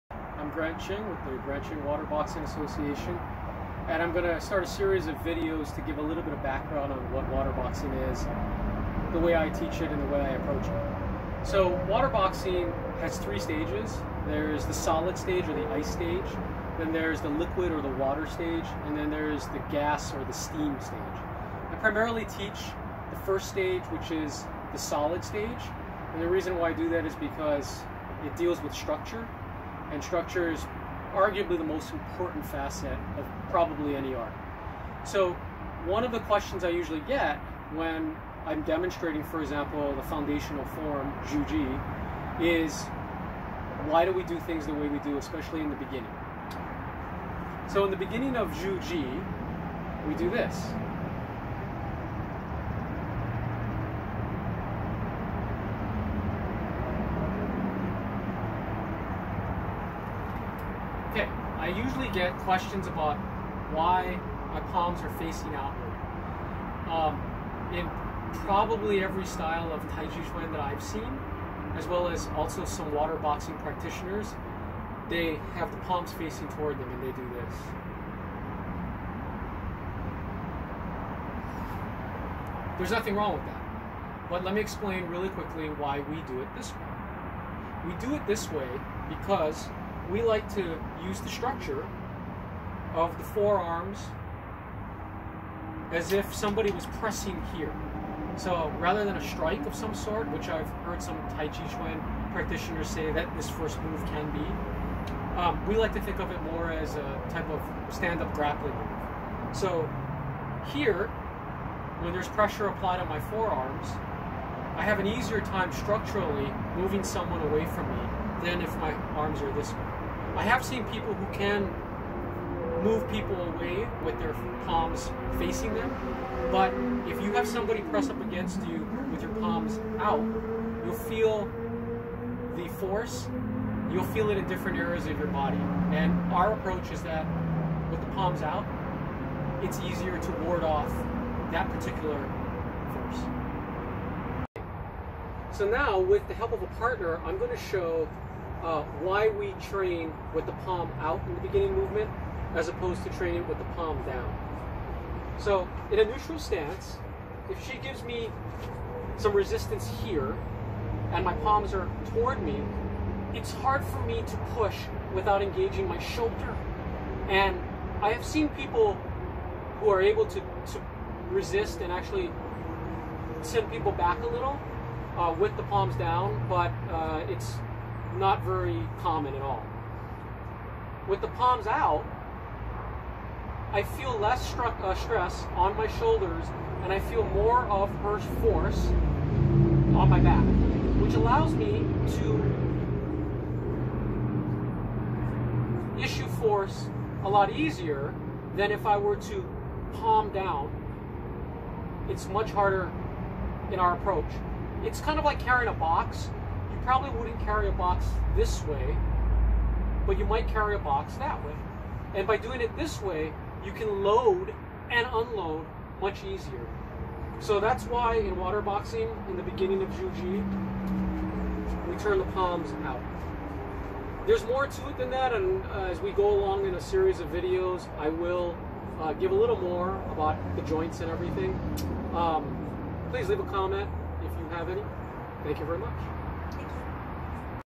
Introduction to Water Boxing (Liuhebafa), Part 1
This is the first in a series of videos introducing water boxing.

Why do we keep our palms out in the opening movement of the foundational form, Zhu Ji?

For more information on water boxing, go to waterboxers.com.